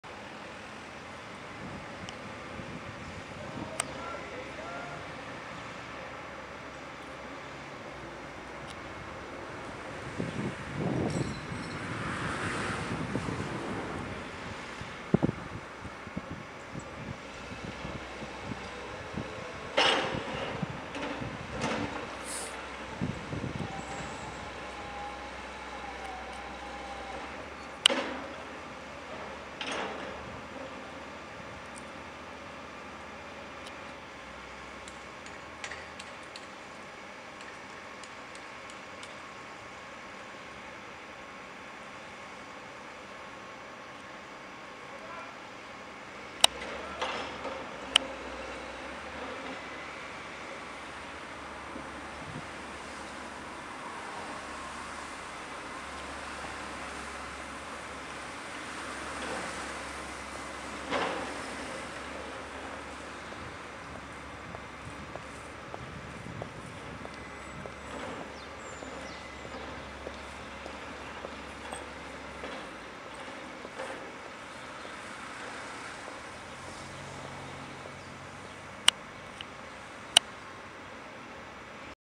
Kettenbagger Caterpillar in Action beim Rammen / Bohren
Kettenbagger Caterpillar BG9H bei Bohrungen auf der Baustelle "Rosengärten" am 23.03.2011 in Berlin-Wilmersdorf